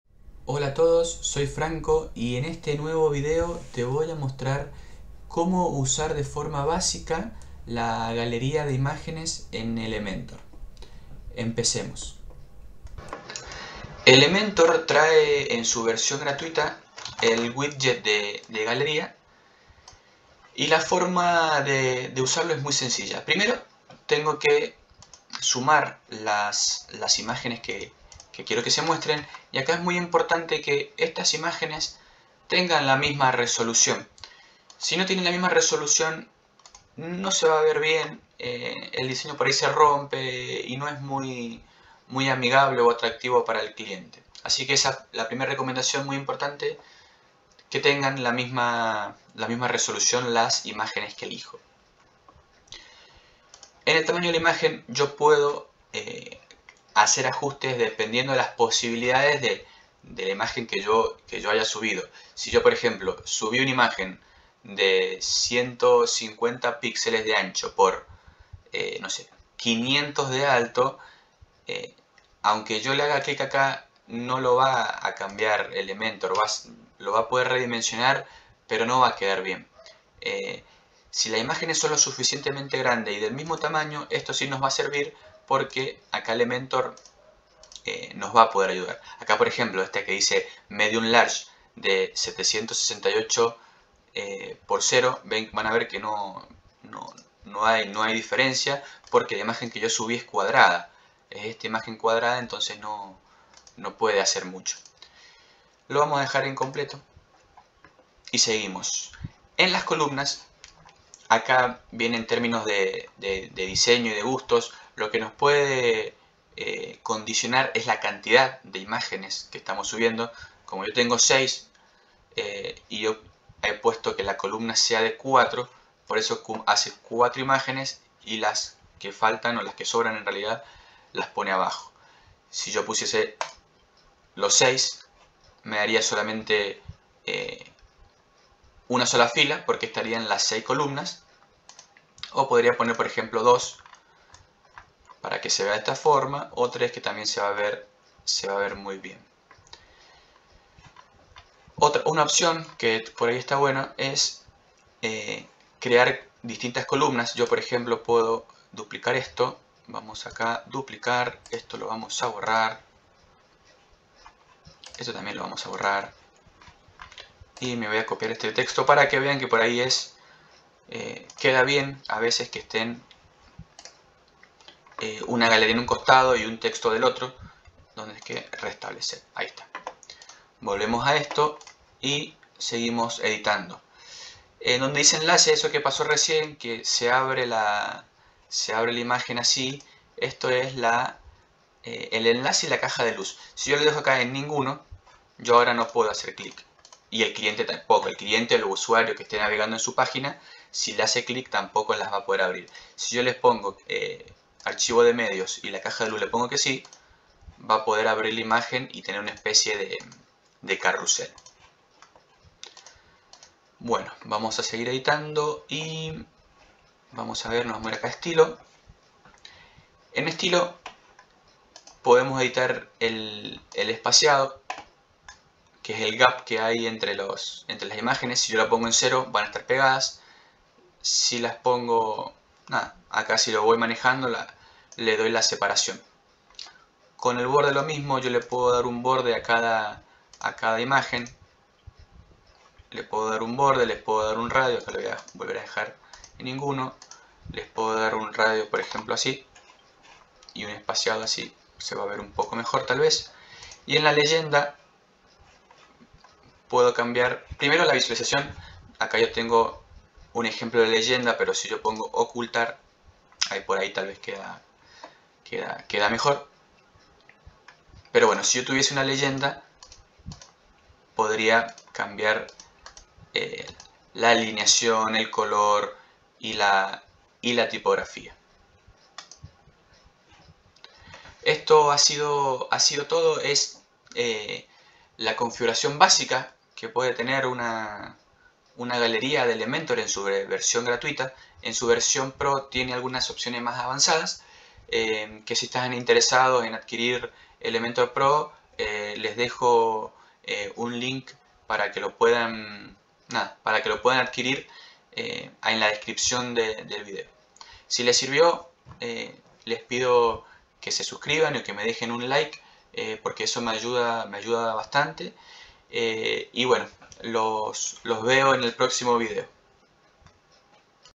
TUTORIAL ELEMENTOR #12 - Así funciona la galería de imágenes 🖼
¡Hola a todos y todas 👋!

En este nuevo video, te mostraré cómo crear una galería de imágenes con Elementor. Con la galería de imágenes de Elementor, puedes mostrar tus fotografías, obras de arte o cualquier otro tipo de imágenes de manera elegante y fácil de navegar.
Agregar una galería de imágenes en tu sitio web con Elementor es muy sencillo. Simplemente selecciona el widget de galería de imágenes de Elementor y arrástralo a la página donde deseas que aparezca. Luego, carga tus imágenes y personaliza el diseño de tu galería según tus necesidades.

Elementor te ofrece muchas opciones de personalización, como el estilo de imagen, la disposición de la galería, el tamaño de las imágenes y mucho más. También puedes agregar efectos de transición, títulos y subtítulos para cada imagen para hacer tu galería más atractiva.
Además, con la funcionalidad de Lightbox de Elementor, tus visitantes pueden ver cada imagen de manera ampliada y detallada sin tener que salir de la galería, lo que aumenta la interacción del usuario en tu sitio web.